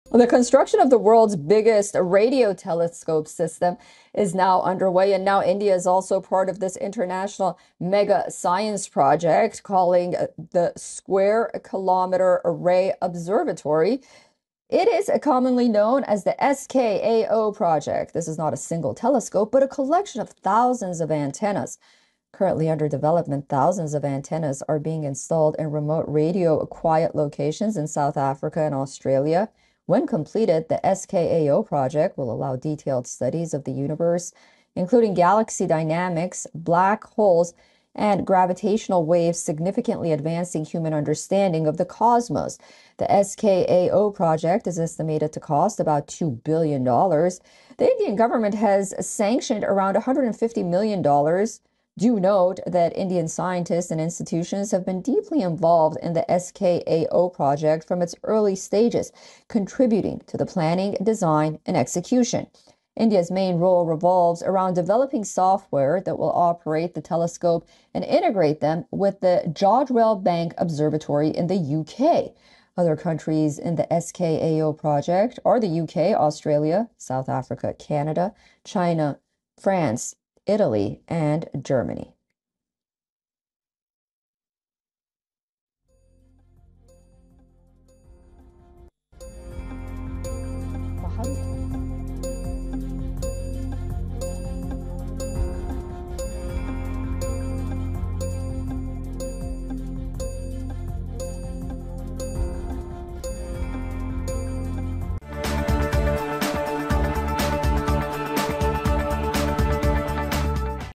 India is now part of world’s largest radio telescope project SKAO | WION
The Square Kilometre Array Observatory (SKAO), an international mega-science project that will serve as the largest radio telescope in the world, will now include scientists in India. This comes as ISRO launched a unique observatory on Monday to study X-rays and black holes in deep space, and plans are underway to build the third LIGO node in Maharashtra.